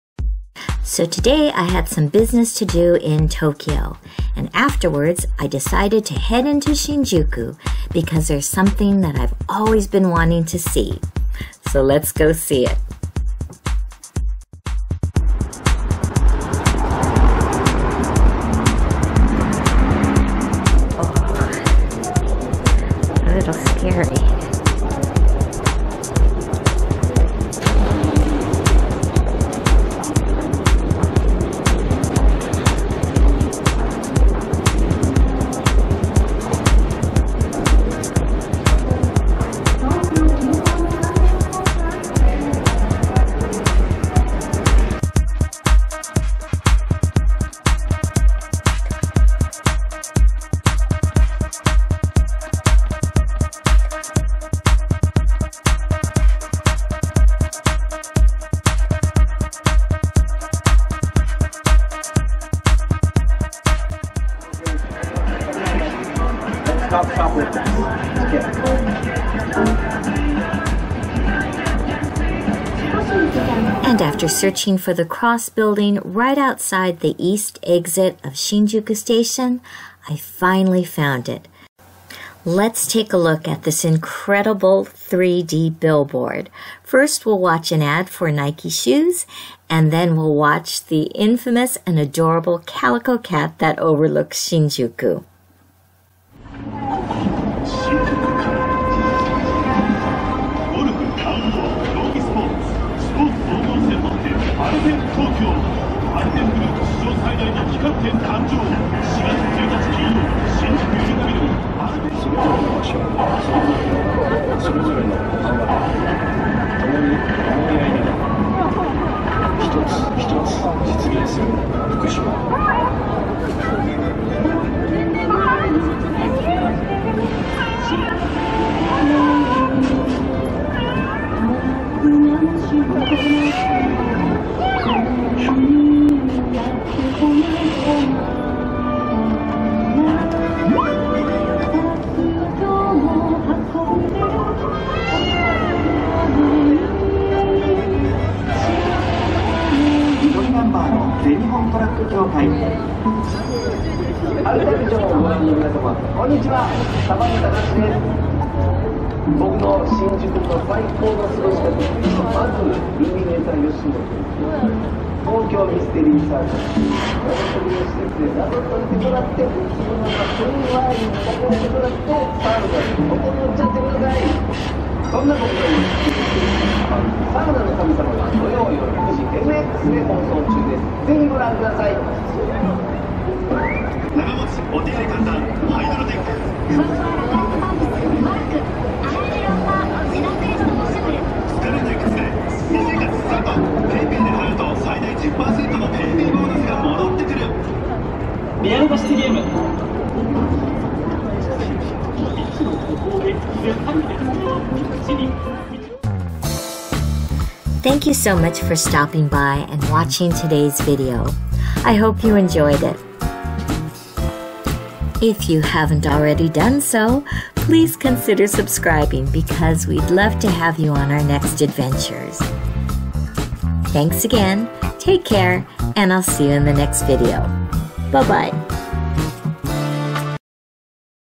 Finally Meeting the Jumbo Calico Cat on the 3D Billboard in Shinjuku
Since its debut in July 2021, people are mesmerized by the Cross Vision 3D billboard in Shinjuku, Tokyo. The billboard currently hosts a Nike ad and also displays messages and announcements. But the true star of this billboard is an adorable Calico cat who watches over Shinjuku, meows at the crowds below, and playfully charms everyone who steps out of the East exit of busy Shinjuku Station. I finally had a chance to see the billboard in person. And yes, I was totally enchanted! What a charming way to take a little break from the hustle bustle of the city!

Thank you, for your help creating thumbnails.